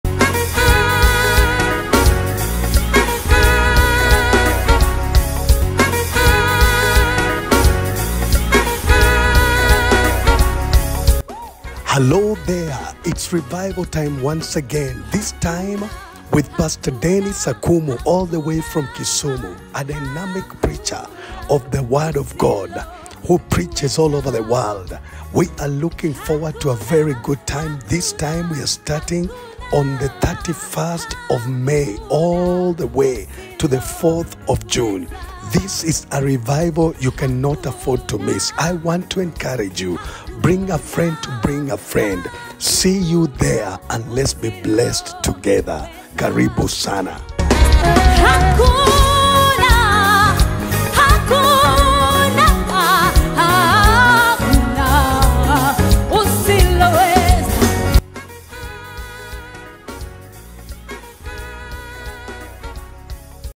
HOPE FOR DIFFICULT TIMES | Revival Services | 31st May - 4th June 2023 | Ps Dennis Akumu
We welcome you to revival services with Pastor Dennis Akumu from our Kisumu church from the 31st of May to the 4th of June. Come experience a move of God in your life. Invite a friend, workmate, relative, neighbour...see you all there!